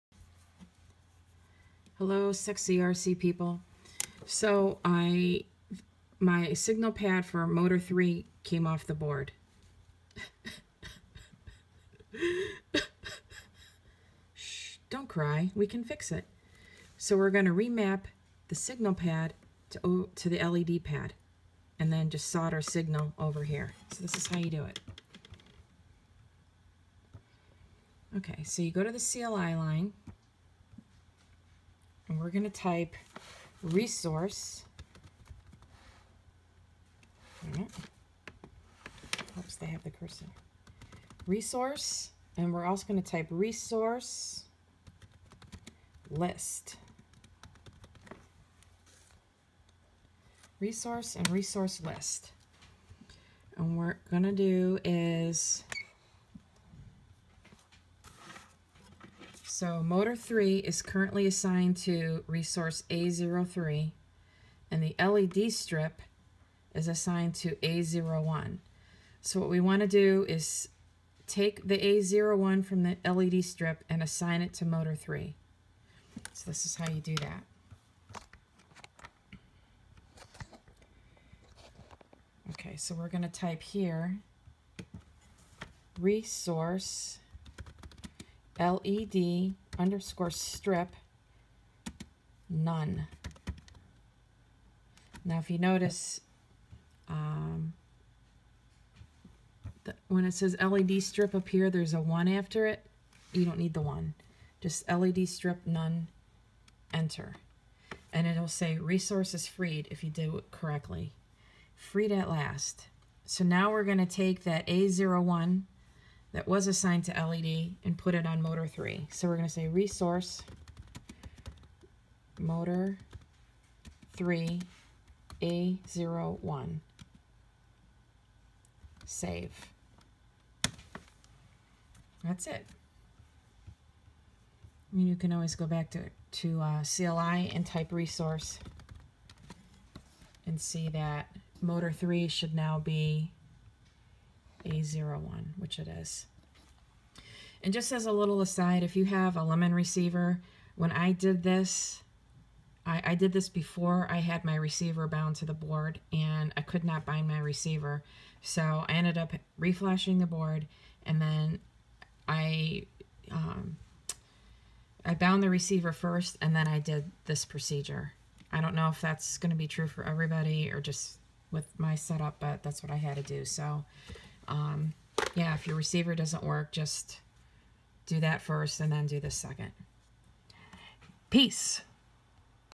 ramapping motor signal pad to led strip pad
If a signal pad comes off the board, remap it to the led pad.